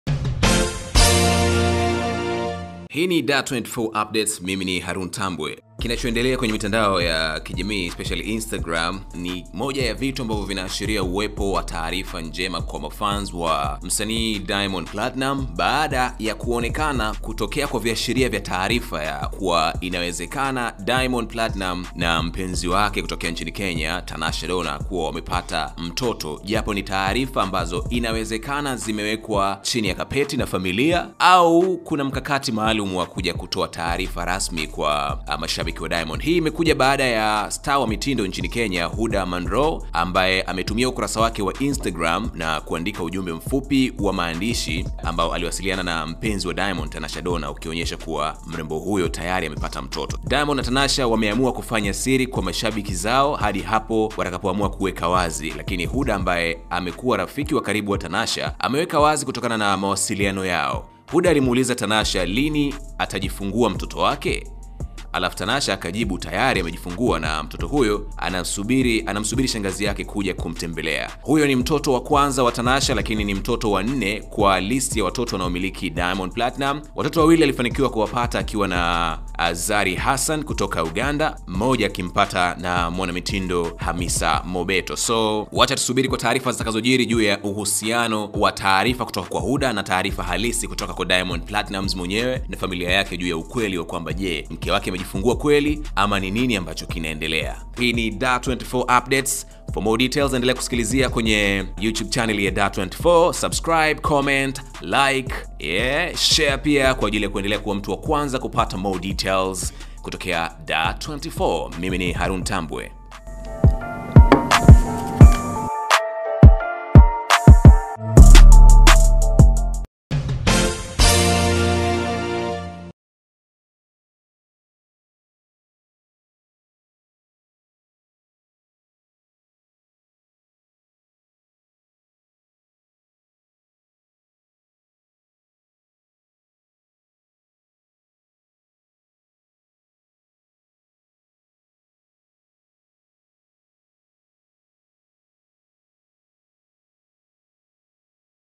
Siri imefichuka mtoto wa Tanasha na Diamond/Huddah afichua kila kitu
Hizi ni taarifa kutoka katika mitandao ya kijamii mara baada ya kuwepo kwa fununu za chini chini za mpenzi wa Diamond Platnumz, Tanasha Dona kuonesha kuwa amejifungua, Dar24 itaendelea kukujuza mara tu baada ya taarifa kuthibitishwa kutoka kwa wahusika wenyewe.